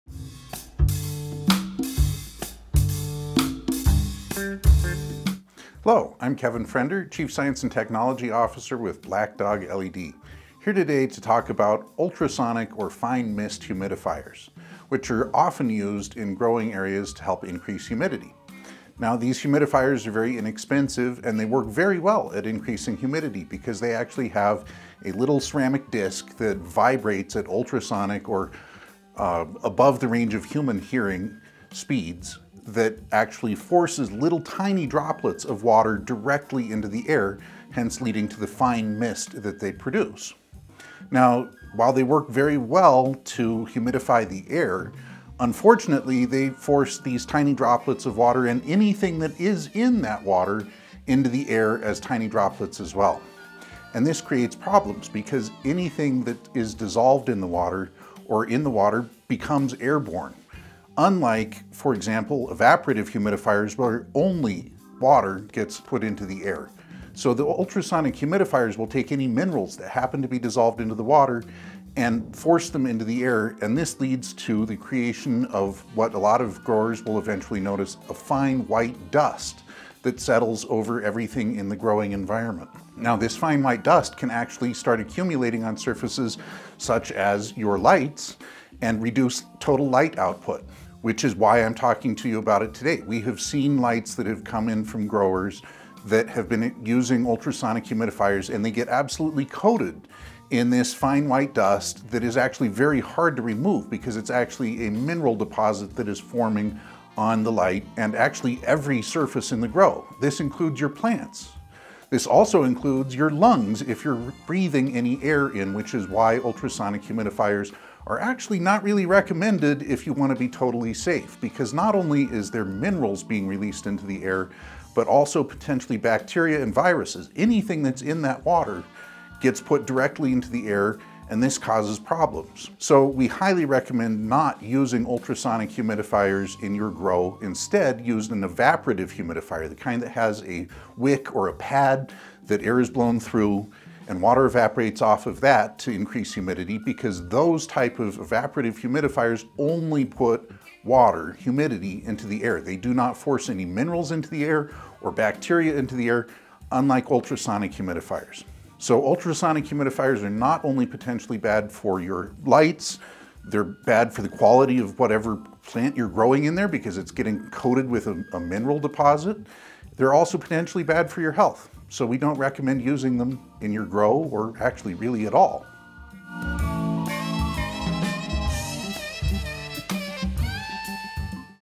Kevin’s Corner – Ultrasonic vs. Evaporative Humidifiers
Ultrasonic vs. Evaporative Humidifiers | The Science of Humidity 🌡️💨

Welcome back to Kevin’s Corner, where we break down the science behind optimal grow environments! Today, we’re diving deep into humidifiers—specifically the battle between ultrasonic vs. evaporative systems. 🌫️⚡

Humidity control = healthier plants, better growth, and maximized yields. But using the wrong system can lead to mineral buildup, mold risks, and inefficient water use. Don’t worry—we’re breaking it all down so you can make the right choice!